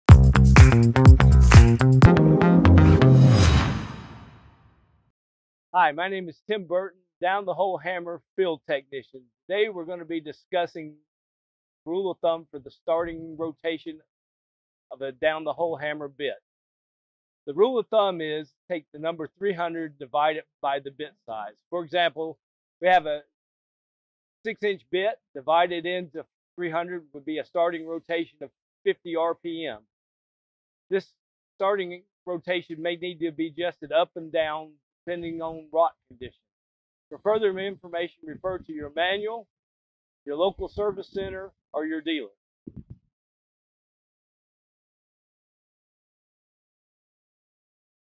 How to find the starting rotation speed | All Epiroc hammers
In this Epiroc 24/7 video, Tim Burton, DTH (down-the-hole) Field Technician, explains how to find the starting rotation speed. Follow along to learn the rule-of-thumb calculation and adjust the starting rotation based on specific rock conditions you may encounter.

Applies to the following models:
All down-the-hole hammers